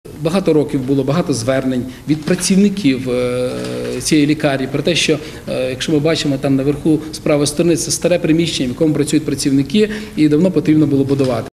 Олександр Пуршага про будівництво корпусу онкодиспансеру
В Чернівецькому обласному онкологічному диспансері триває будівництво нового двохповерхового корпусу відділення патоморфологічних досліджень та реконструкція двох поверхів лікувального корпусу – реанімації та відділення хіміотерапії.